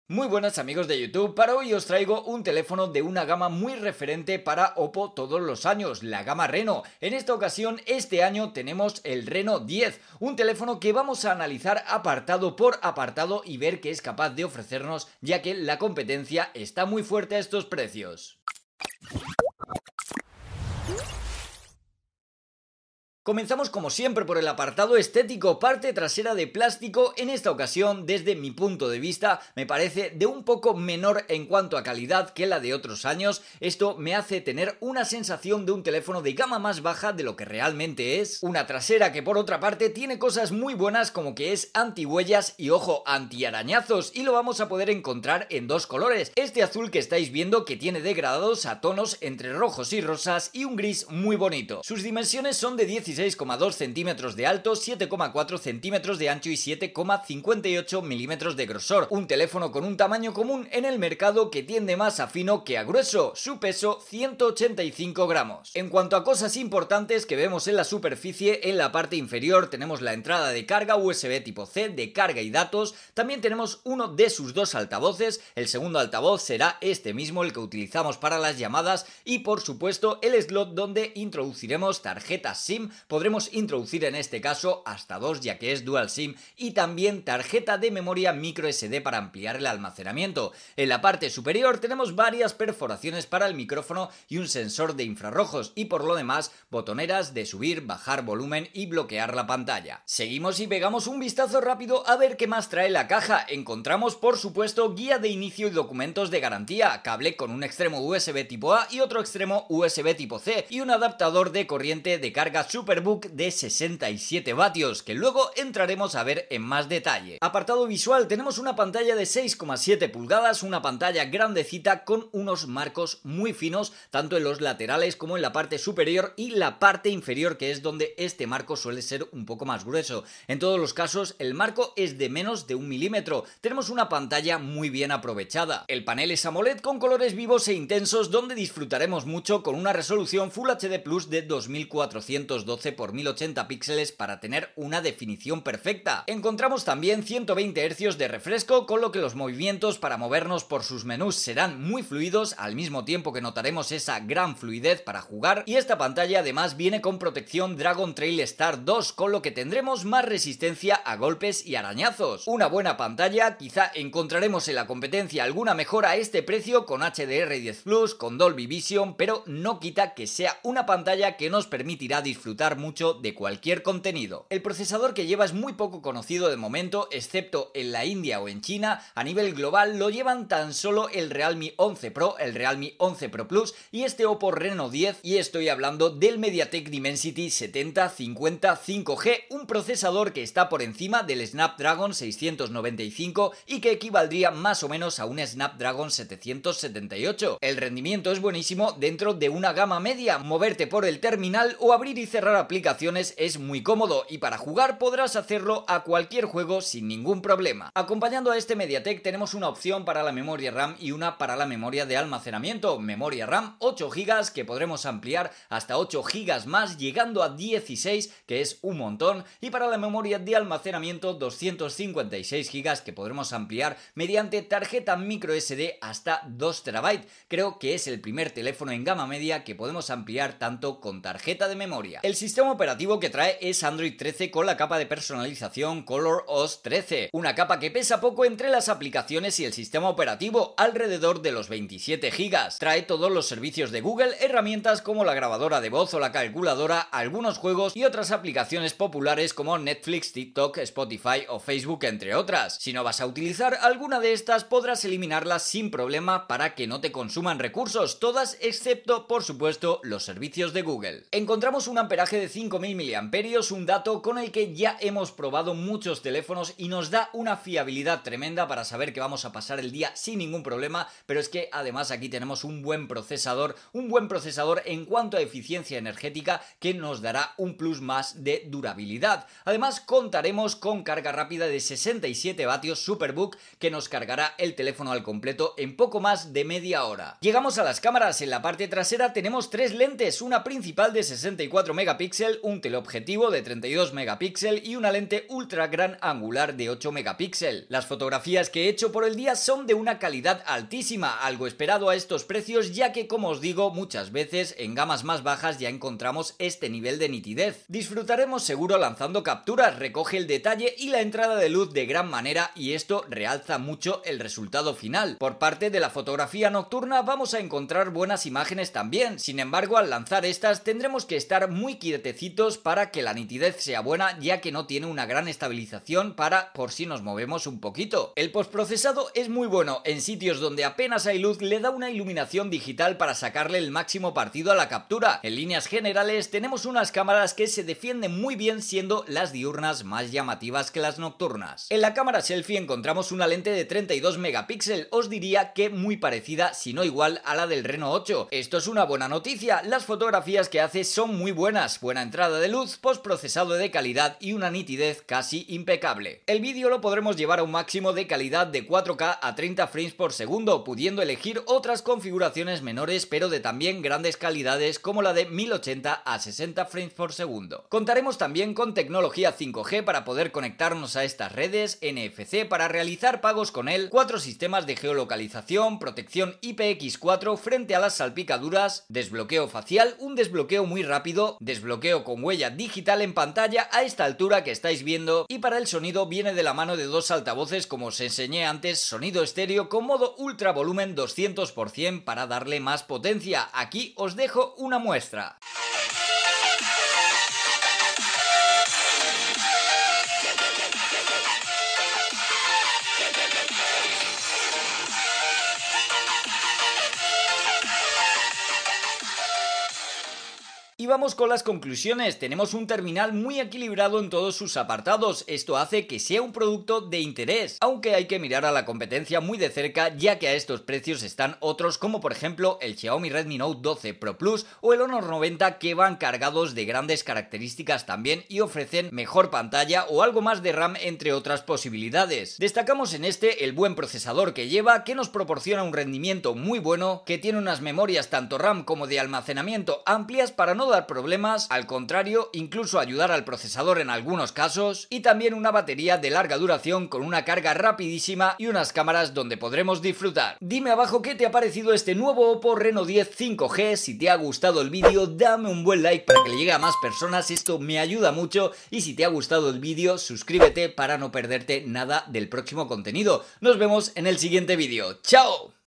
Oppo RENO 10 5G - ¿El teléfono QUE ESPERABAS?☝
En este vídeo os enseño y analizamos el Oppo Reno 10 5G , un teléfono del que esperamos mucho puesto que es una referencia en gama media todos los años.

No os lo perdáis , además de ser un teléfono muy bonito tiene unas cualidades que seguro os van a sorprender.

OPPO RENO 10 5G

Muchas gracias a tod@s!